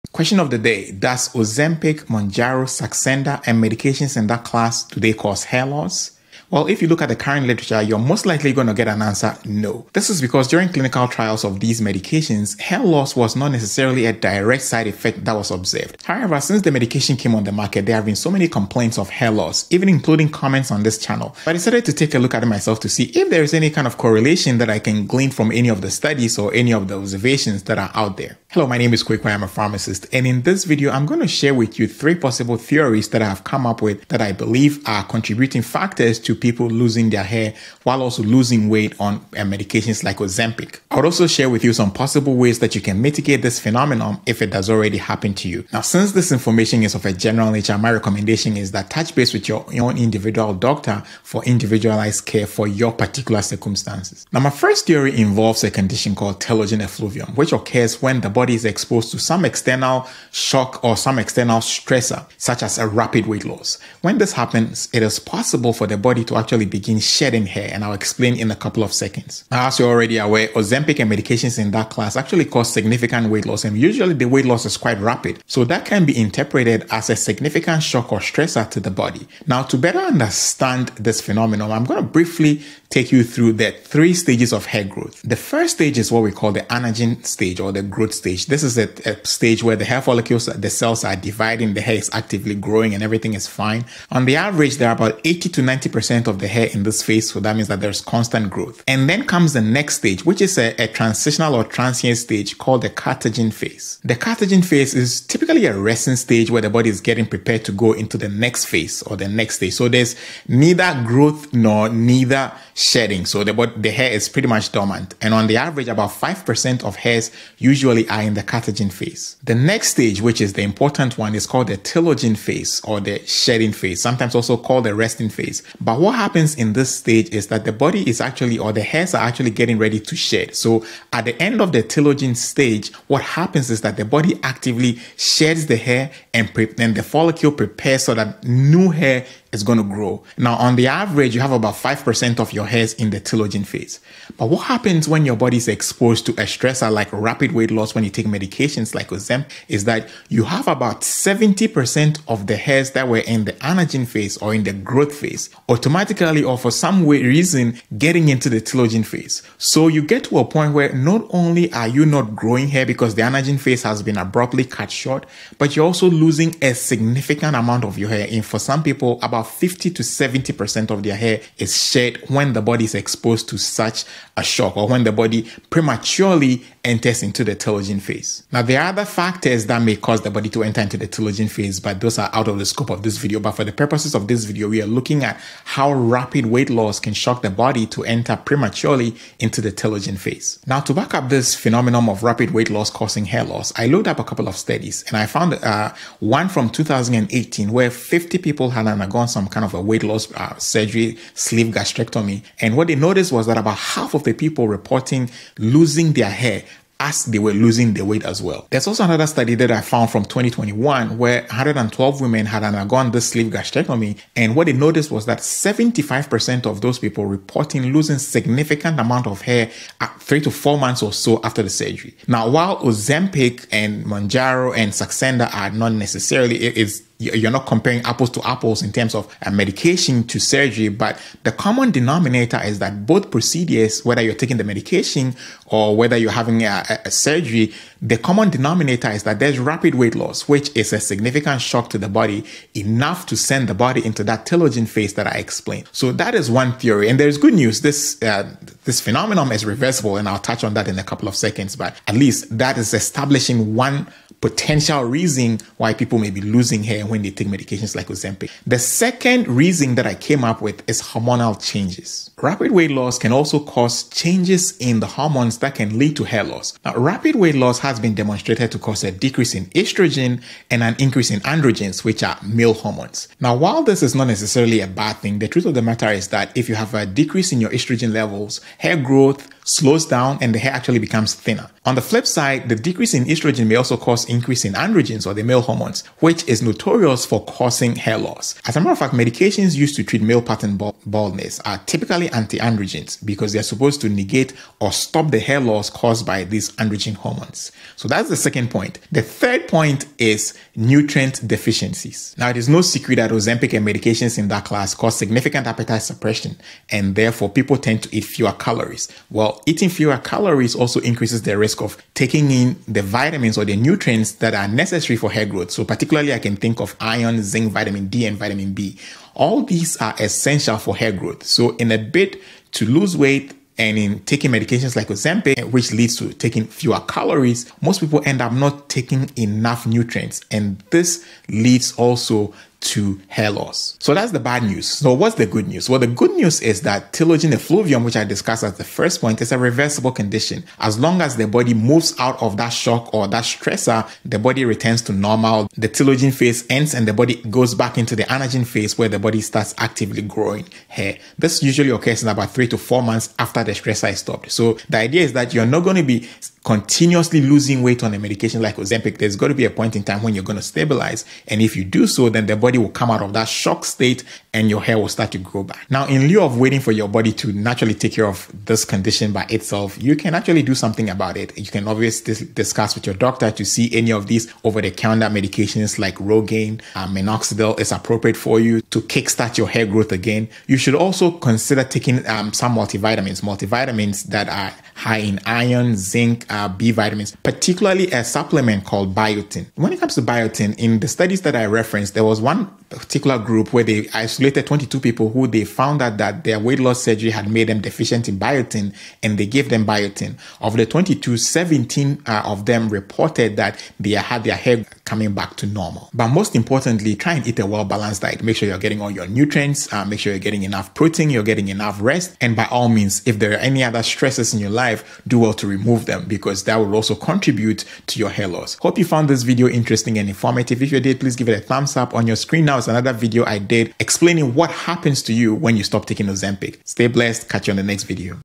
"Hair Loss Alert! Find Out What Ozempic, Mounjaro & Saxenda Could Be Doing to Your Hair..."
Does Ozempic, Mounjaro, Saxenda, Wegovy, etc. cause hair loss?

A video addressing the correlation between rapid weight loss for people taking medications like Ozempic (Semaglutide), Mounjaro (tirzepatide), Saxenda (liraglutide), and hair loss. The video addresses the possible causes and ways to mitigate this phenomenon.

Some Hair Loss/Growth Products You May Consider:
Rogaine For Women
Rogaine For Men